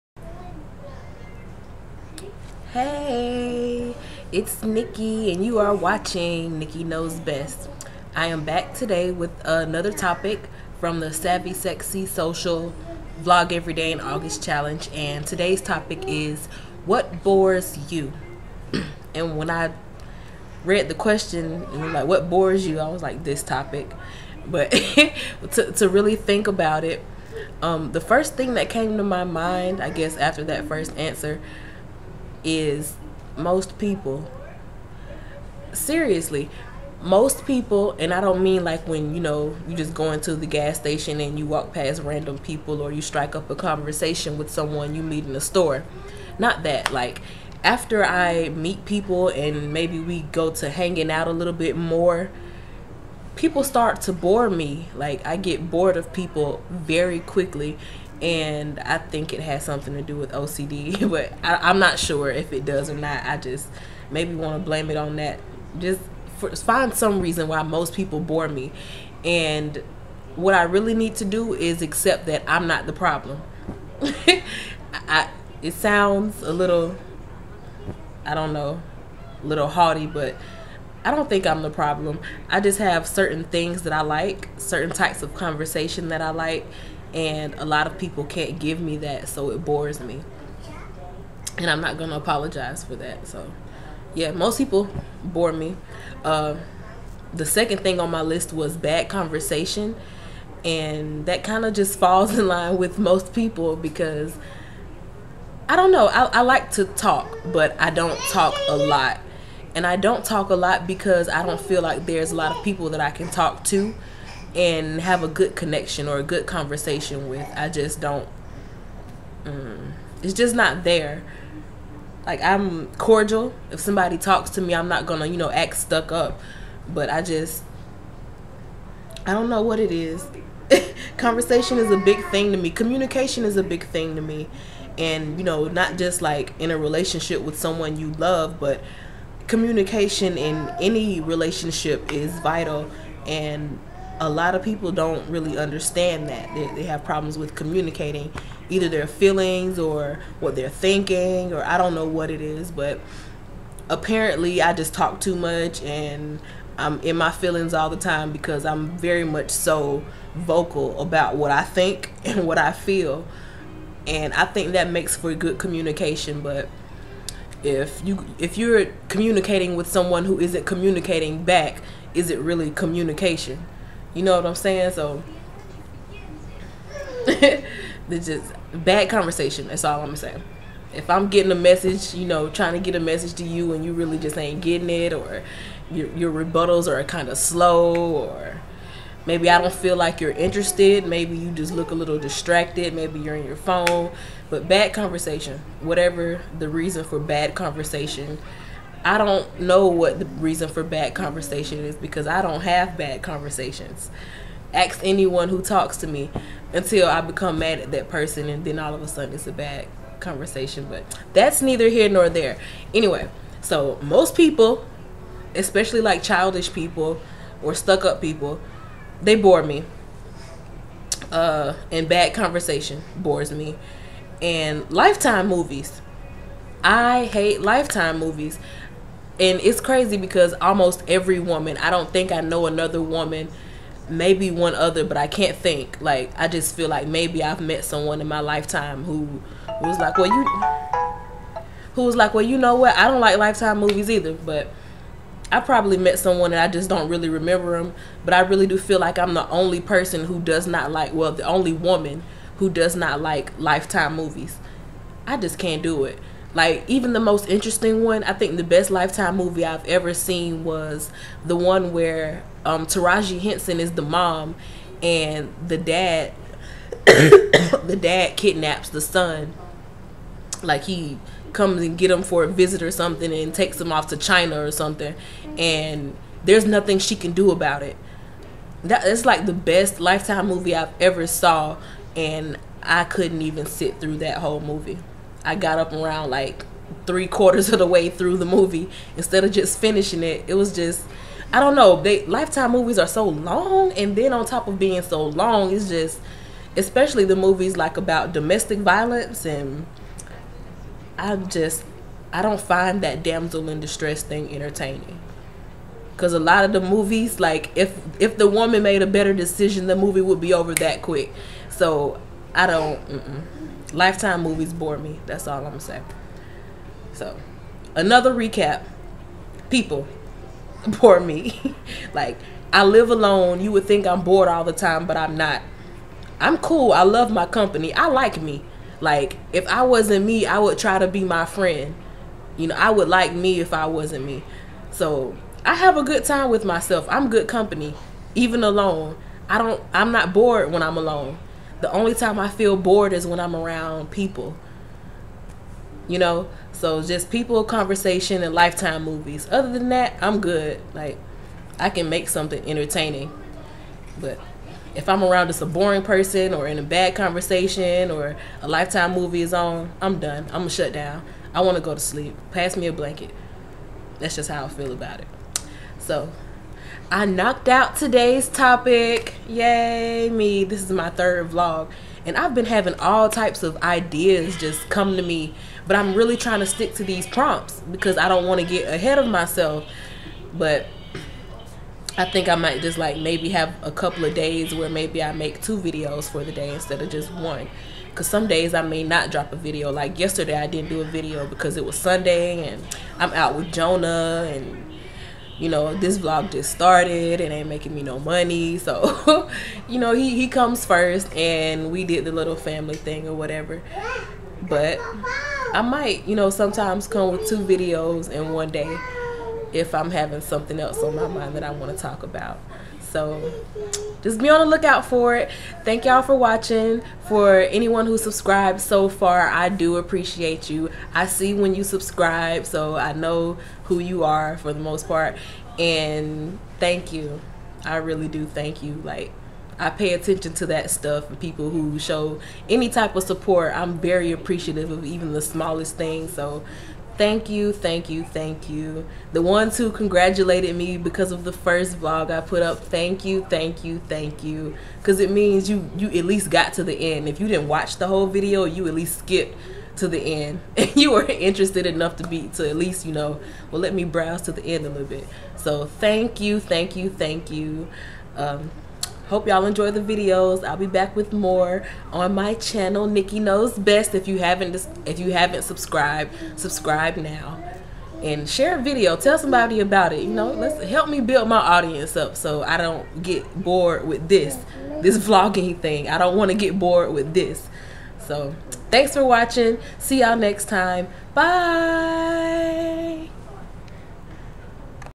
[Vlog #3] - Most People Bore Me...
Nikki shares the things that make her yawn in this video for the SSSVEDA2014 challenge. Enjoy. :)